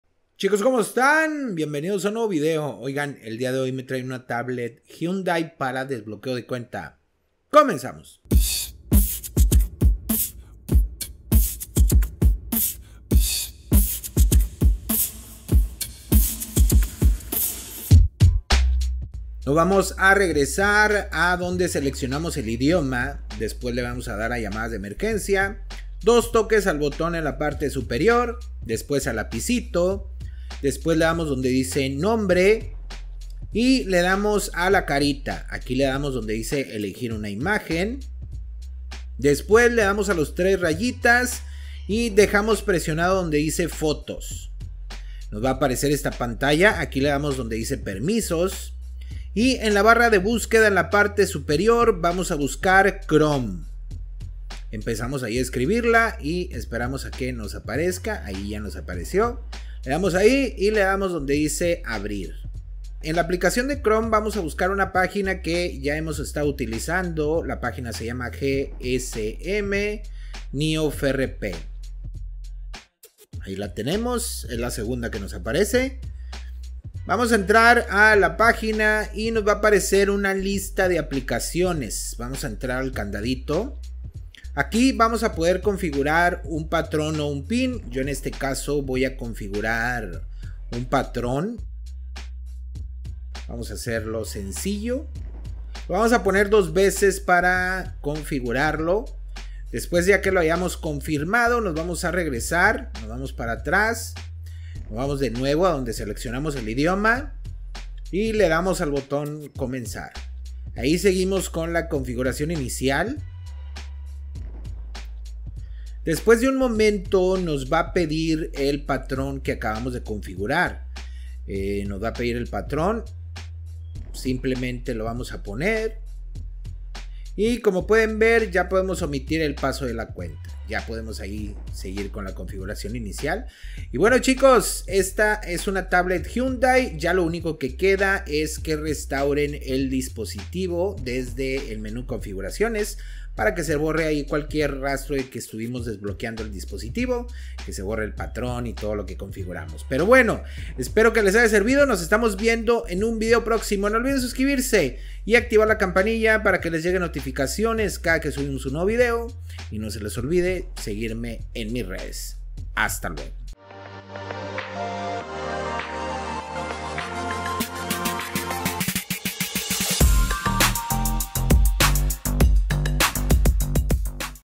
COMO DESBLOQUEAR TABLET HYUNDAI *HyTab Pro*
El dia de hoy me llega una Tab let Hyundai para desbloqueo de cuenta
espero que les sirva